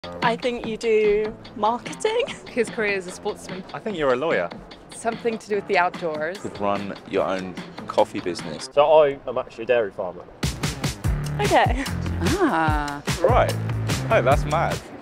Can you guess my career?
Did you guess? Watch people's first reactions here.

Arla is encouraging Gen Z to consider a future career in farming.

As a major employer and a vital business helping to feed the nation, Arla Foods UK understands that we need to attract the next generation of farmers to help deliver future changes.